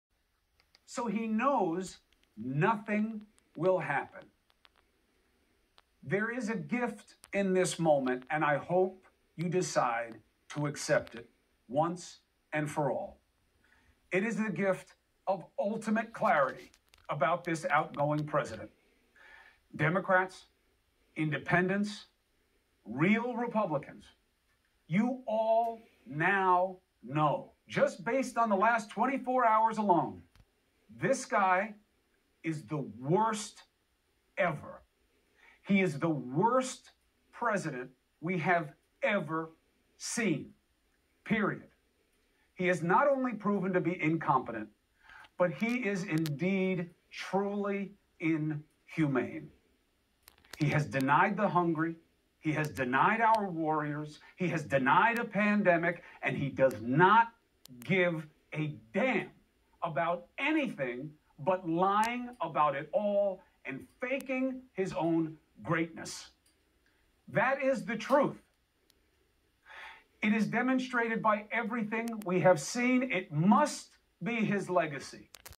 the worst ever
Cris Cuomo goes off on President Trump 110%